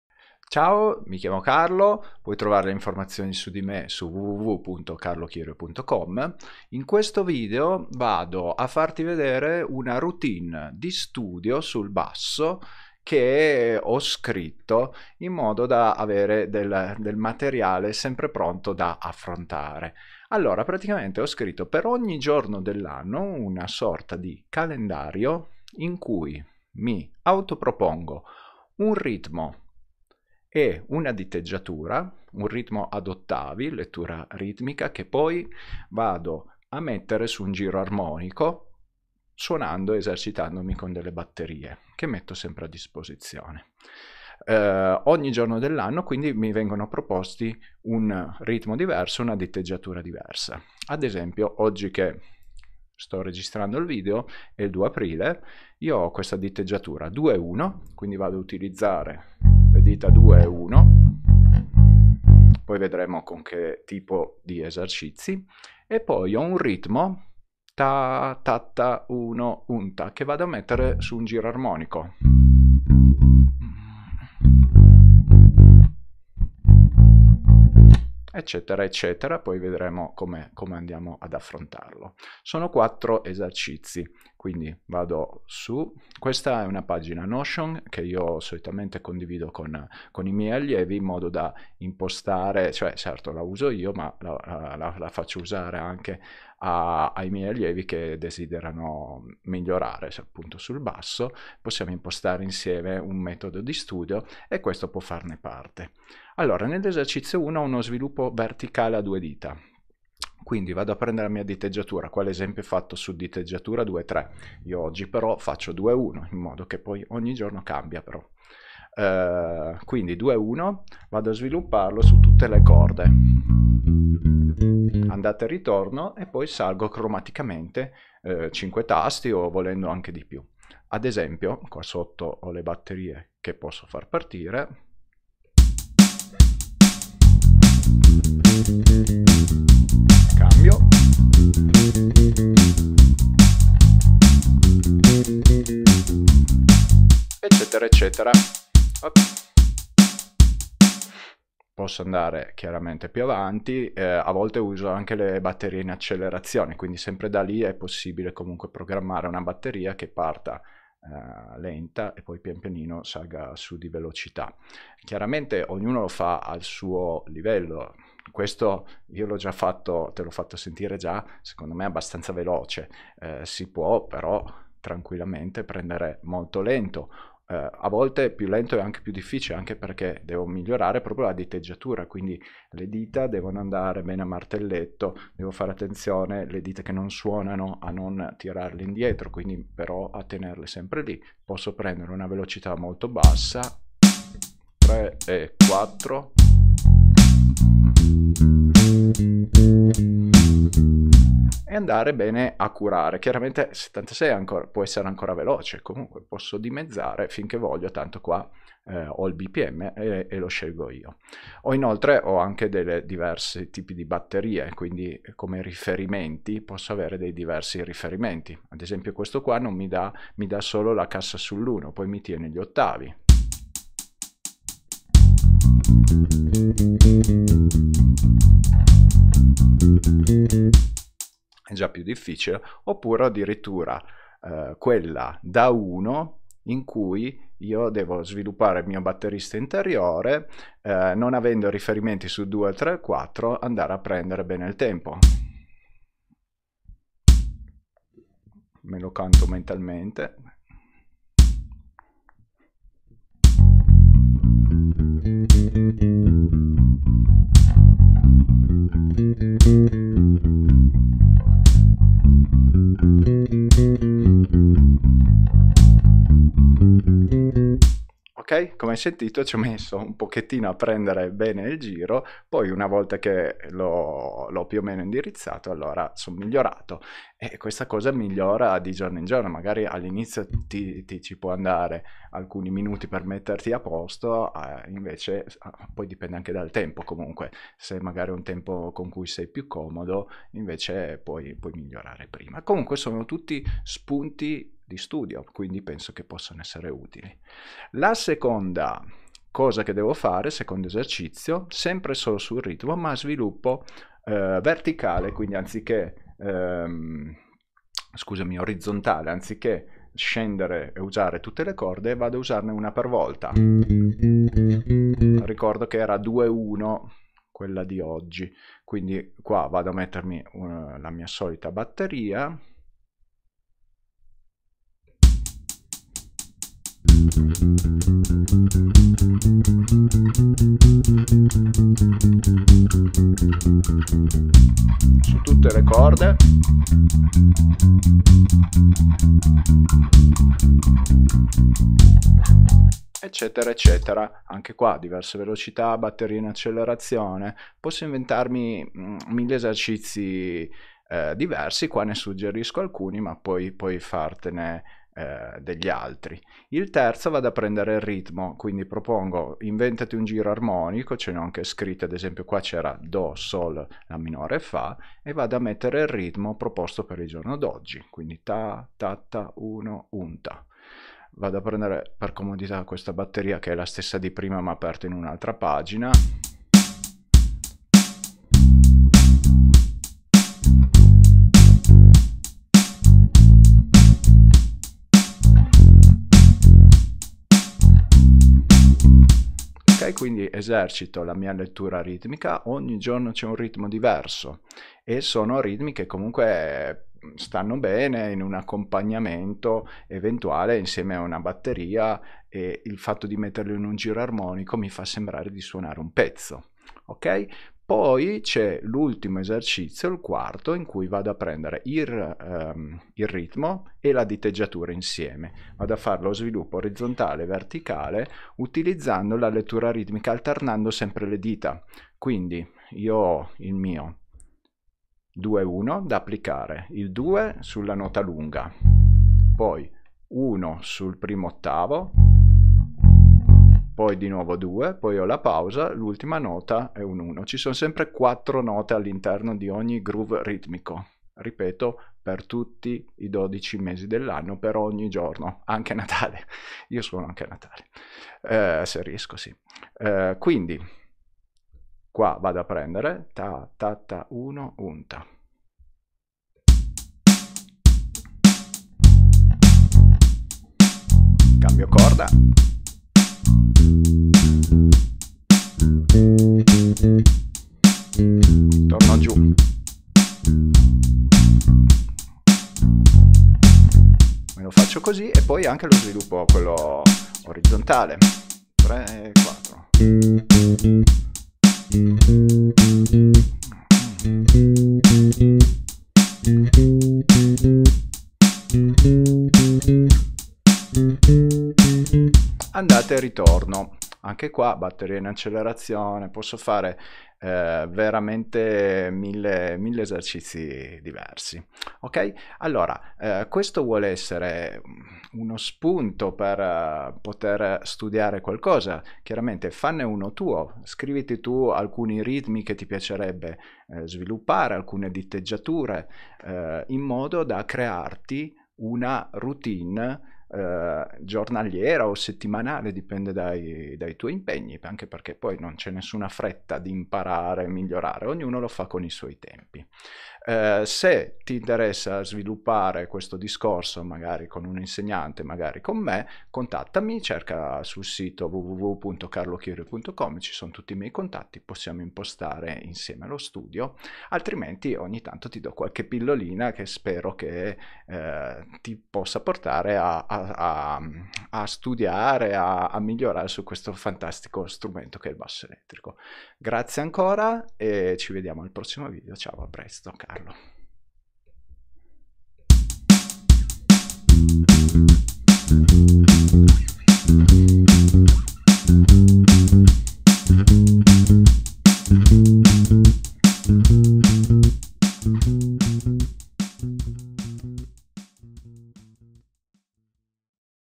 Routine sul Basso: 1 ritmo ed 1 diteggiatura diversi ogni giorno
In questo video ti mostro una routine che mi sono creato per studiare ogni giorno 1 ritmo ed 1 diteggiatura diversi sul Basso elettrico.